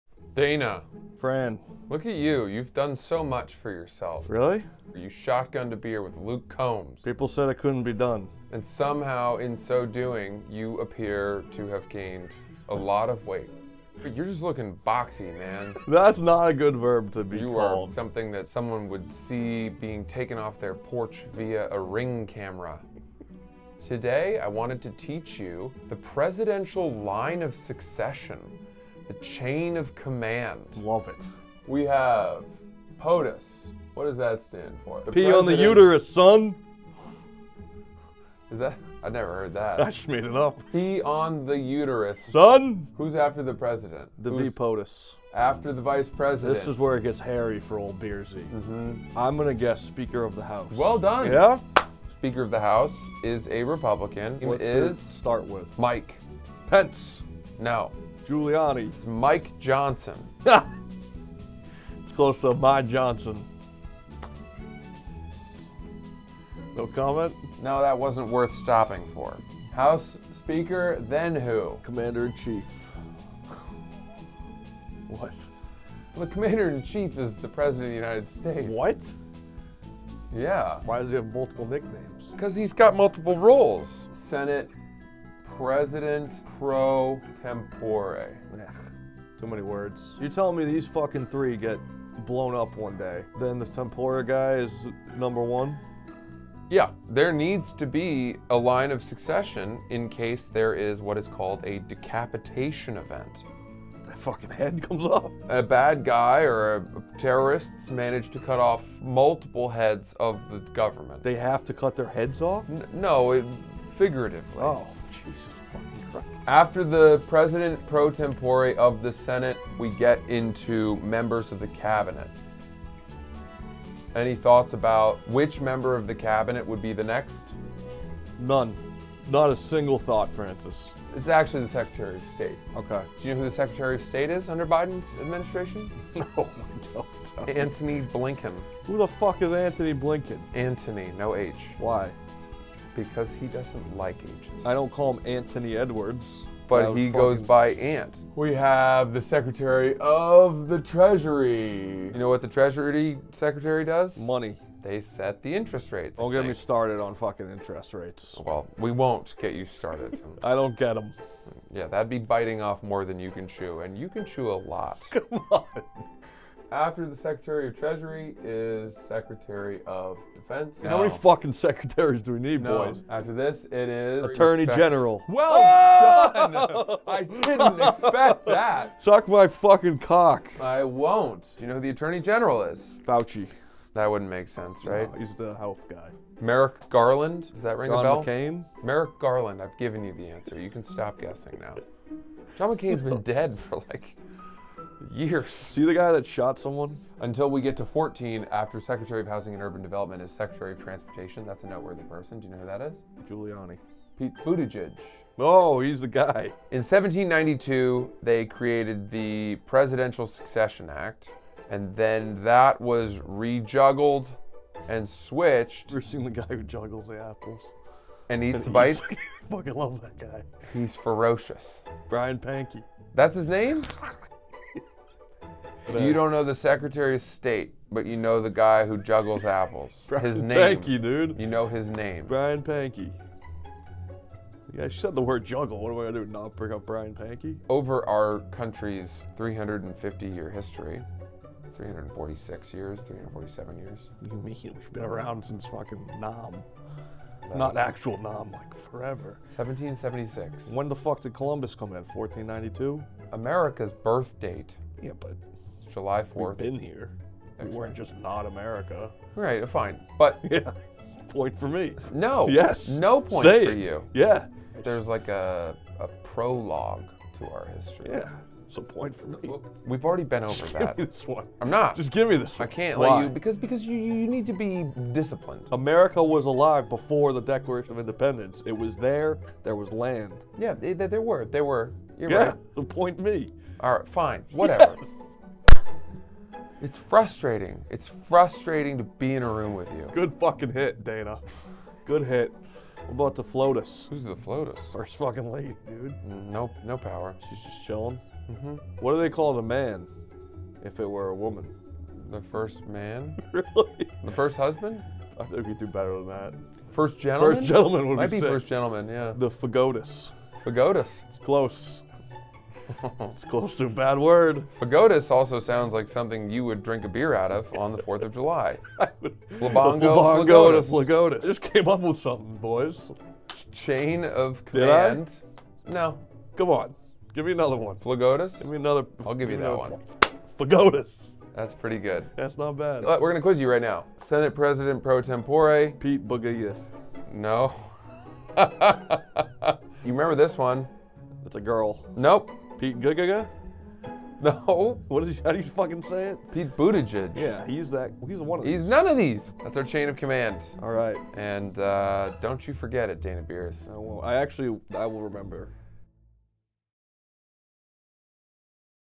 Learning The Presidential Line Of Succession
This seems relevant right now. Let's talk chain of command.